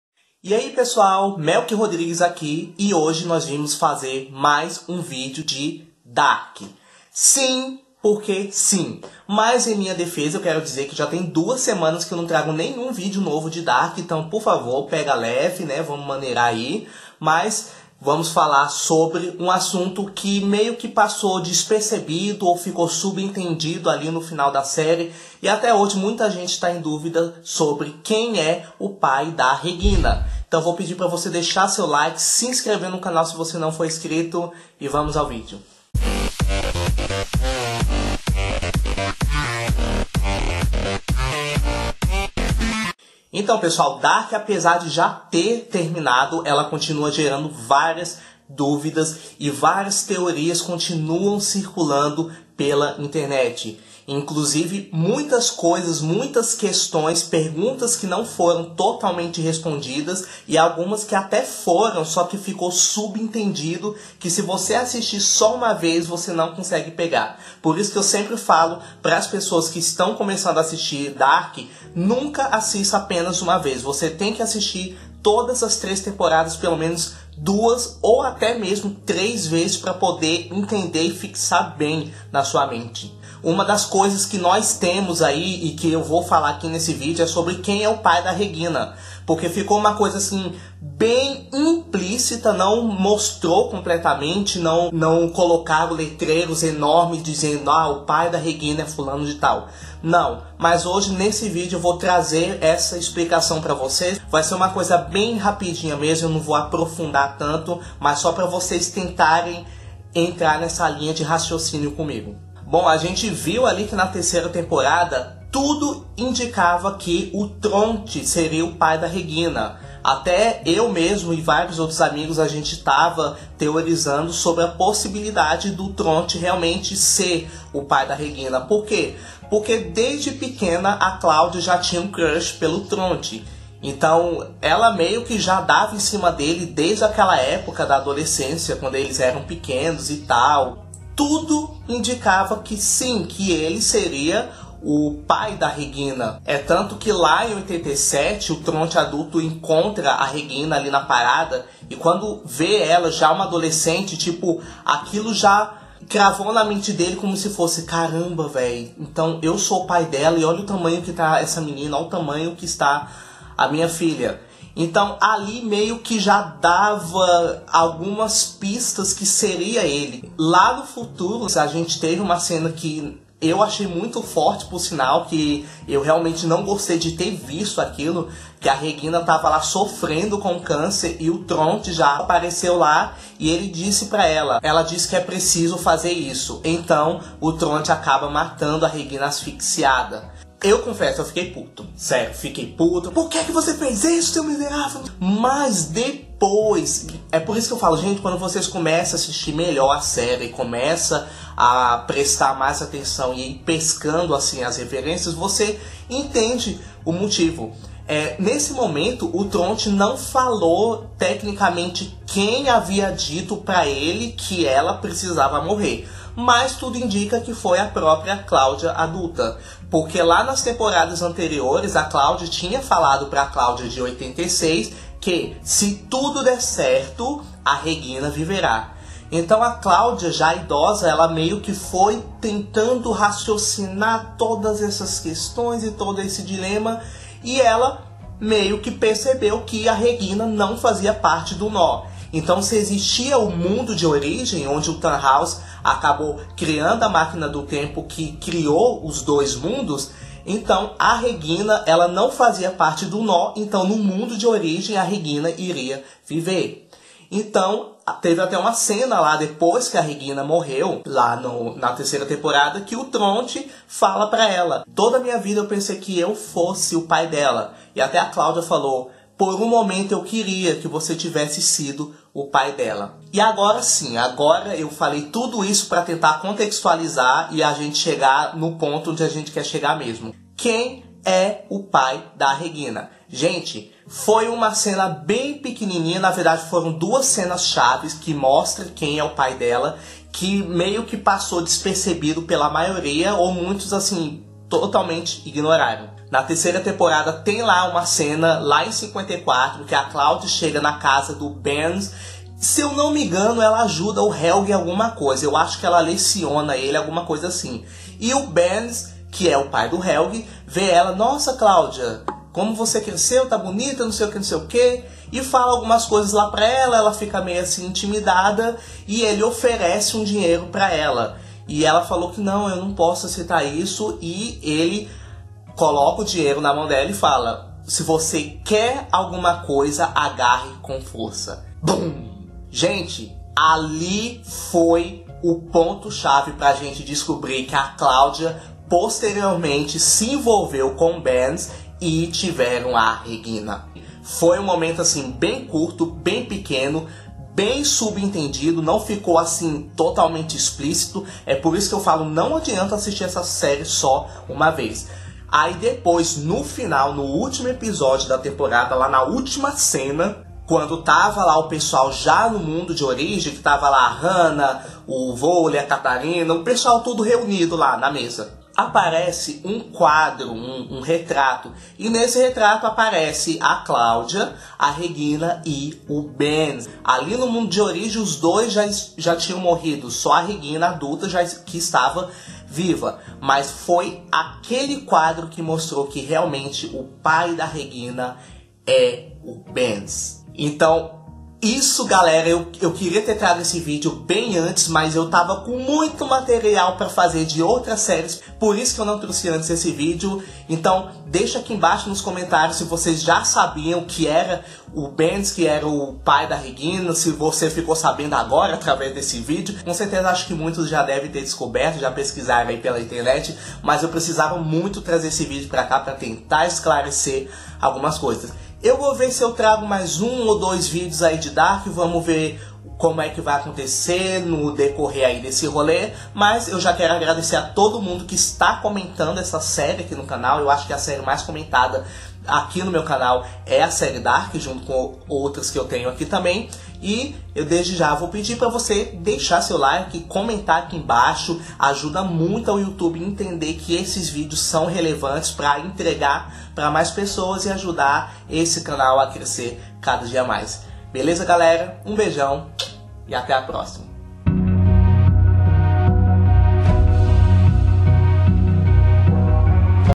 DARK 3 | QUEM É O PAI DE REGINA?
Fala, seus lindos! Hoje retorno com mais um vídeo de Dark e vamos discutir sobre quem é o pai de Regina, que ficou meio subentendido ali no final da série.